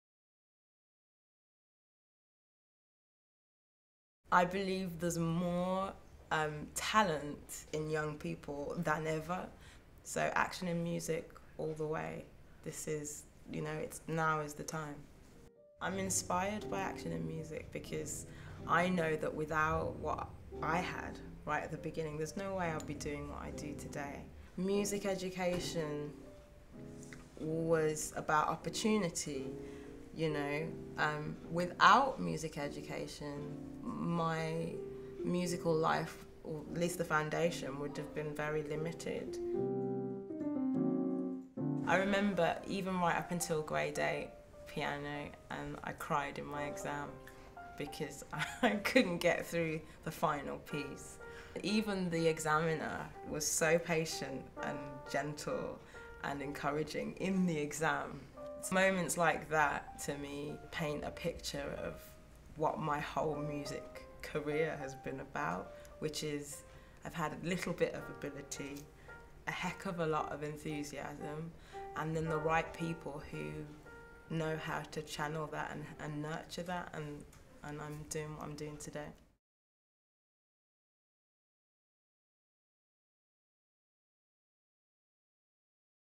Laura Mvula at Evelyn Grace Academy
Looking back at her own education in music, Laura Mvula visited the Evelyn Grace Academy in Brixton where she used to be a pupil to launch the ActionInMusic campaign in schools and encourage pupils to nominate the teachers that have inspired them to learn more about music.